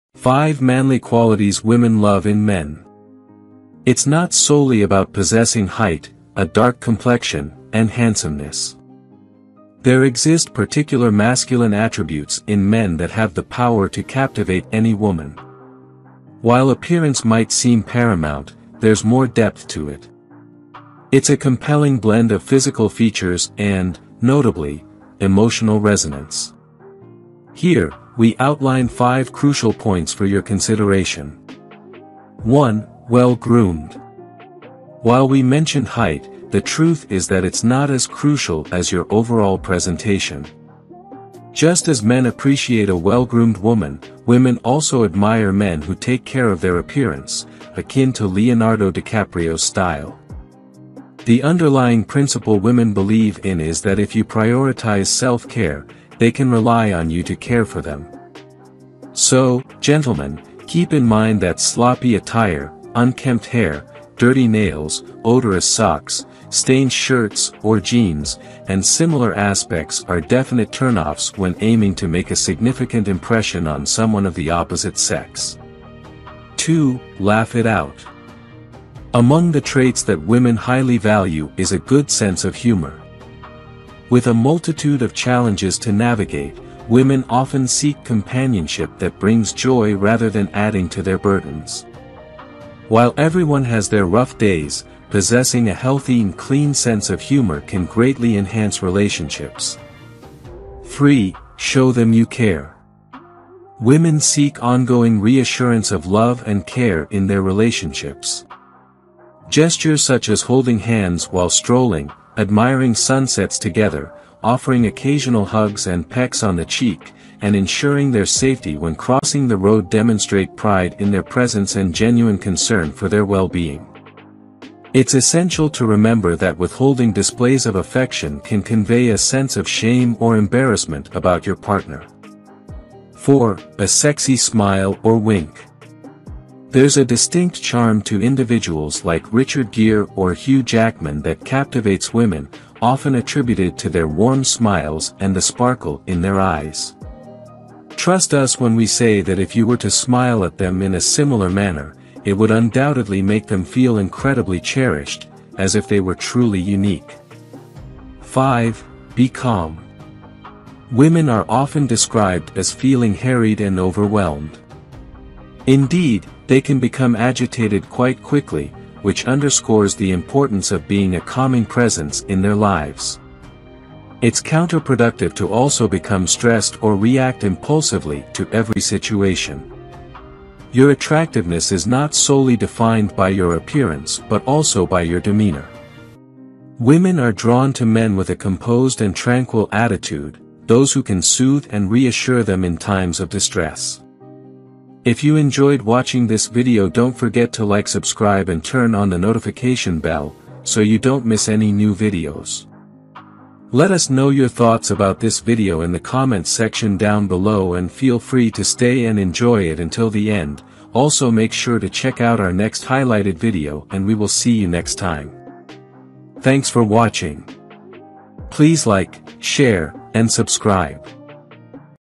5 Manly Qualities Women Love In Men | Psychological Facts About Human Behaviour | Awesome Facts
5 Manly Qualities Women Love In Men | Psychological Facts About Human Behaviour | Awesome Facts

It's not solely about possessing height, a dark complexion, and handsomeness. There exist particular masculine attributes in men that have the power to captivate any woman. While appearance might seem paramount, there's more depth to it. It's a compelling blend of physical features and, notably, emotional resonance. Here, we outline five crucial points for your consideration.
1. Well Groomed:
2. Laugh It Out:
3. Show Them You Care:
4. A Sexy Smile Or Wink:
5. Be Calm: